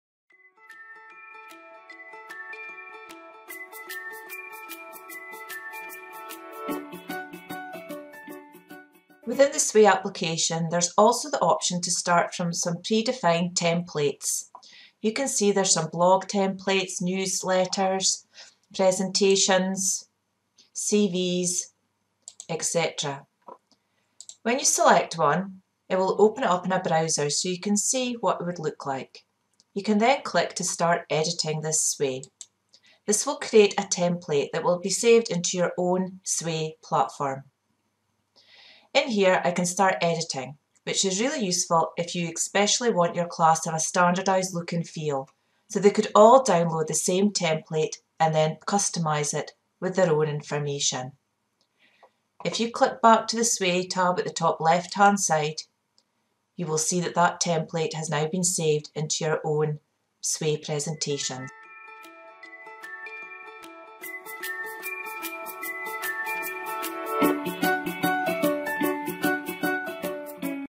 What is Sway? Creating a Sway from a Template
Bitesize video about creating a Sway from a template. This video is part of the 'What is Microsoft Sway?' bitesize video collection.